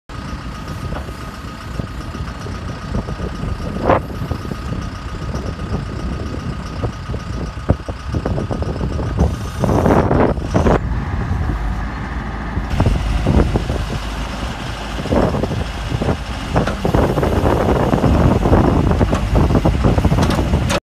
2008 DEERE 310SJ For Sale
Hydraulic Excavator, EROPS, 4x4, Extend-A-Hoe, Pilot Controls, Plumbed, Mechanical Coupler, Solideal 19.5-24 Rear Tires-50%, Firestone 12.5/80-18 Front Tires-10%, 7'6" GP Bucket, 18" Rear Bucket, (No Air in Cab), Shows: 5,178 Hours